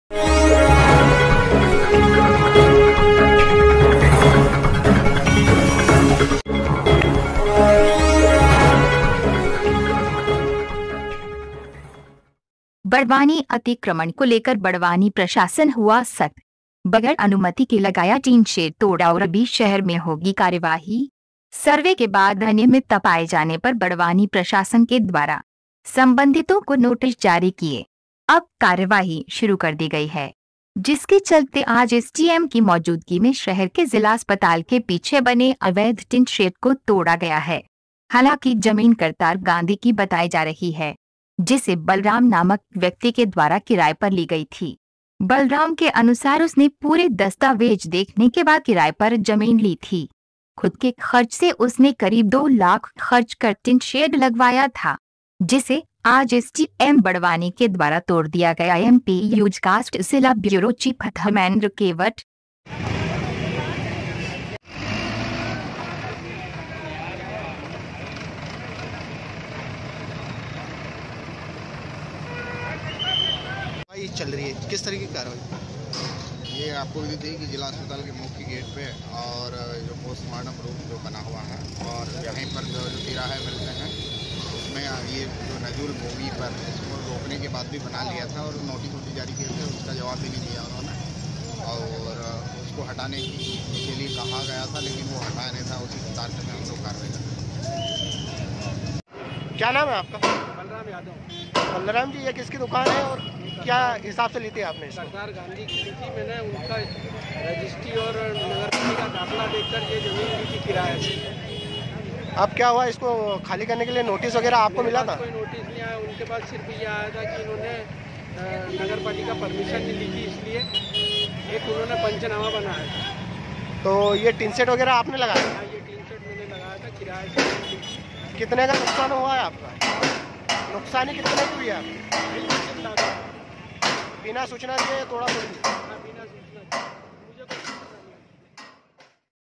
बड़वानी -अतिक्रमण को लेकर बड़वानी प्रशासन हुवा सख्त बगैर अनुमति के लगाया तीन शेड तोड़ा ओर भी शहर में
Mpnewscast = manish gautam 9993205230